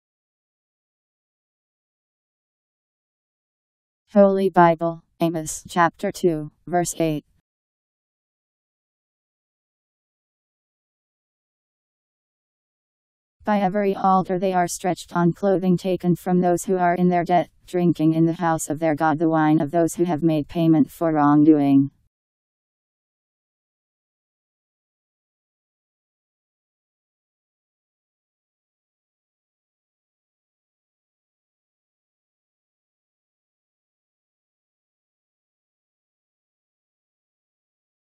Holy Bible Amos 2:8
Video shows Holy Bible Amos Chapter 2 Verse 8: By every altar they are stretched on clothing taken from those who are in their debt, drinking in the house of their god the wine of those who have made payment for wrongdoing. Powered by MaryTTS. Text taken from Bible in Basic Edition. Bible Amos 2:8 Bible Learning, what Bible verse says.